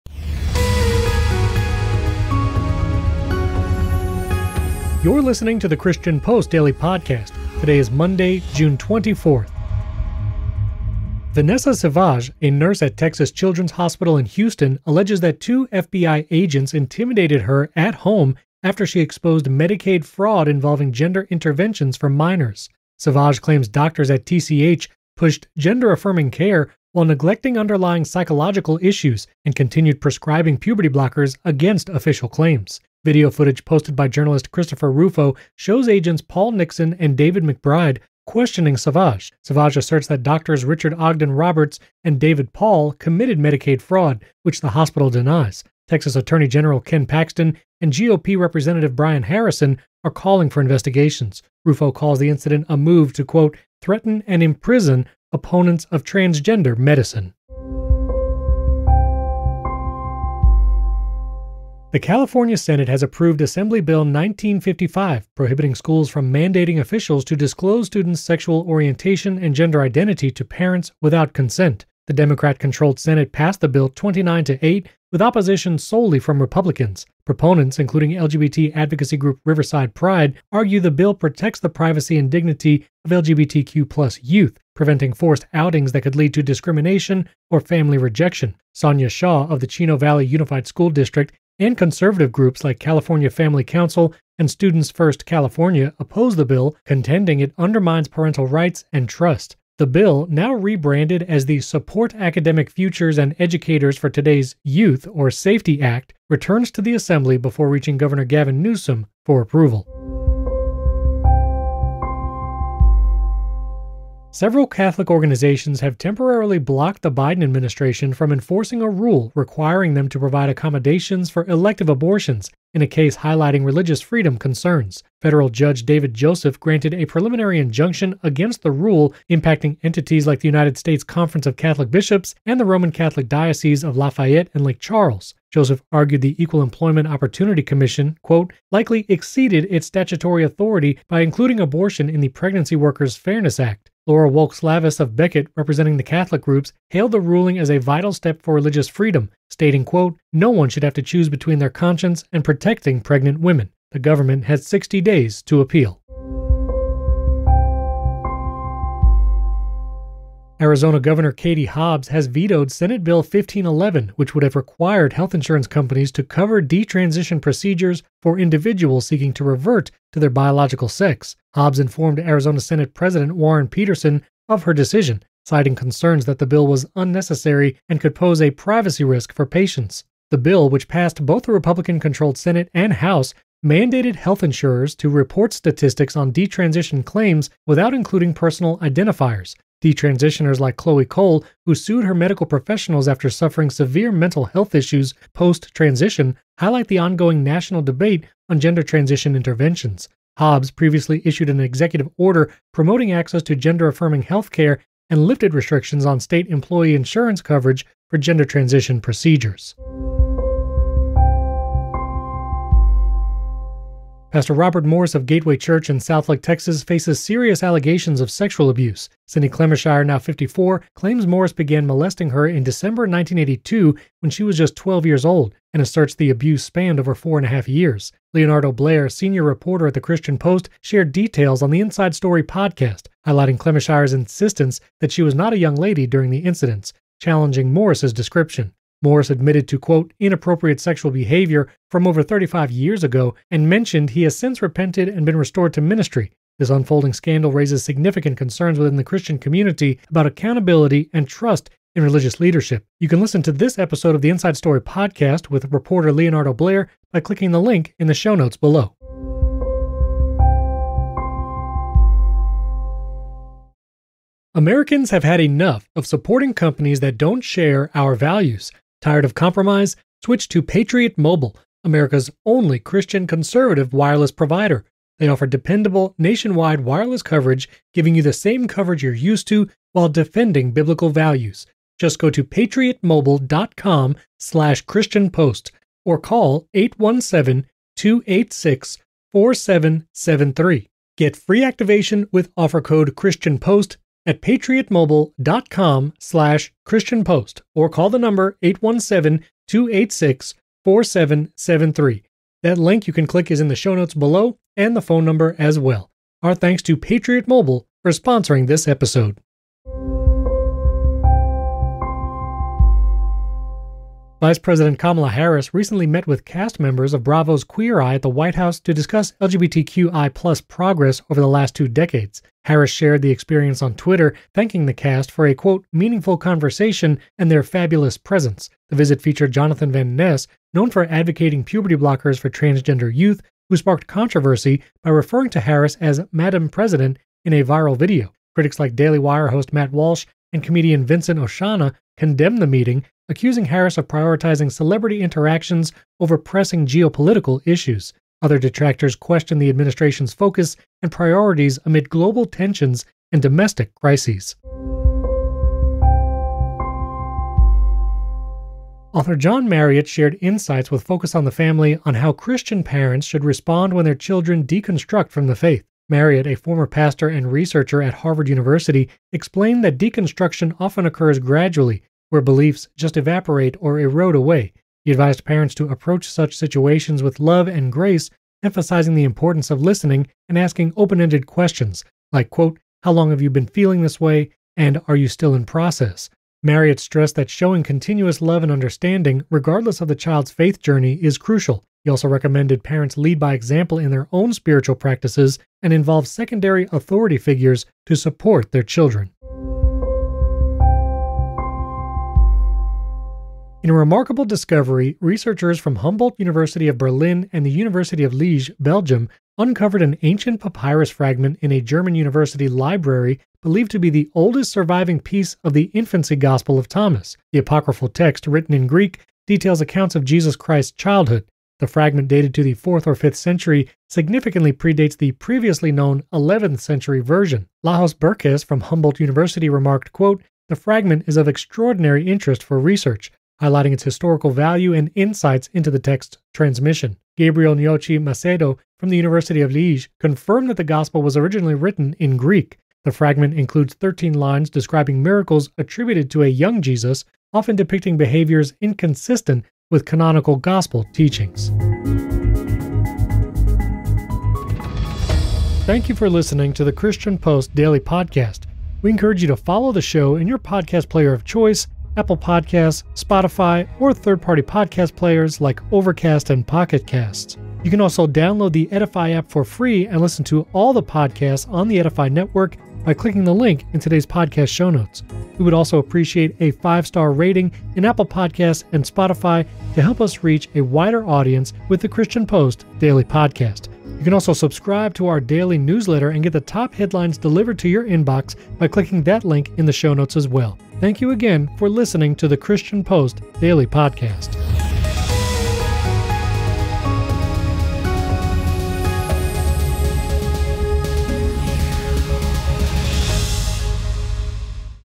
CA Senate Bans Parental Notification, What to do When Children Deconstruct, FBI Threatened Hospit...
Top headlines for Monday, June 24, 2024

In today’s episode, Vice President Kamala Harris hosting Queer Eye cast sparks social media backlash, California Senate's new bill on social transitions in schools, and a critical discussion with a relationship expert on guiding children through faith deconstruction. Plus, a judge's ruling supporting Catholic organizations against a Biden administration mandate on employee abortions.

⠀Get the Edifi App

⠀Links to the News

• Kamala Harris draws scorn for hosting 'Queer Eye' cast | Politics News
• Calif. lawmakers pass ban on school parent notificaiton mandates | Politics News
• What should Christian parents do when their children deconstruct? | Living News